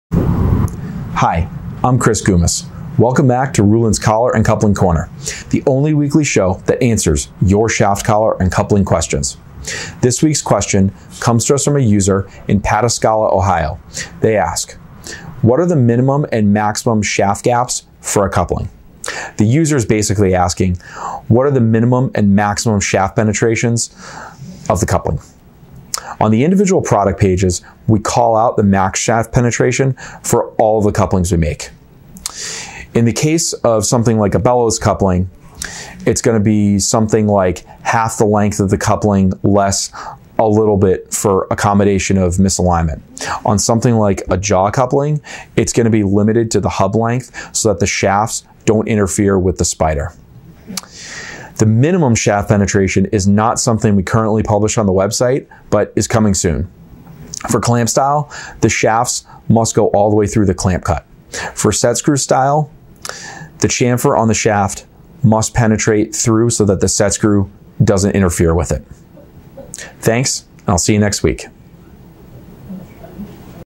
What Are The Minimum & Maximum Shaft Gaps for a Coupling?
Welcome to Ruland's Collar and Coupling Corner! This is the only weekly show that answers your shaft collar and coupling questions!

This week's question comes to us from a user in Pataskala, OH, who asks, "What are the minimum and maximum shaft gaps for a coupling?"